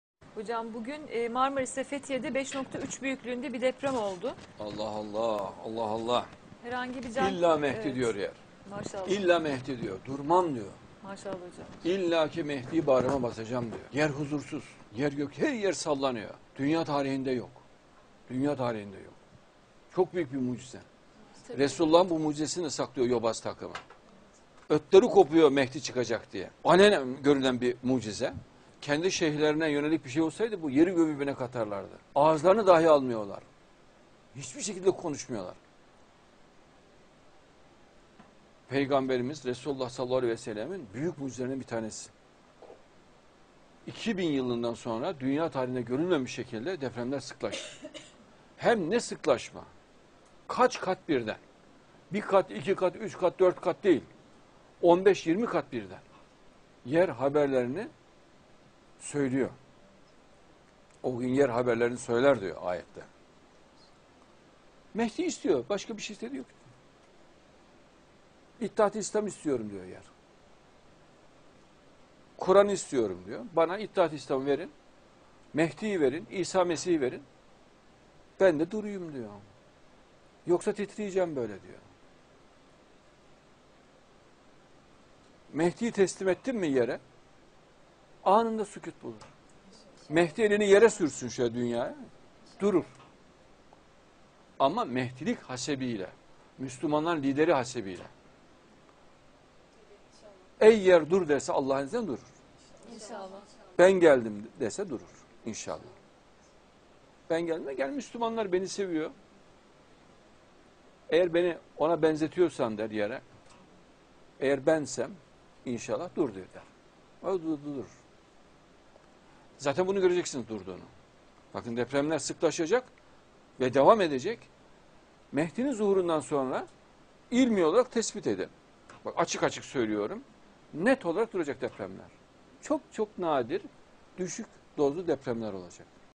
Hz. Mehdi elini yere sürdüğünde yer sakinleşir.
Belaların çözümü Mehdiyettir, Mehdiyeti istemekten geçer. Mehdiyet KurtuluşReçetesi 'dir hem de tüm dünya için...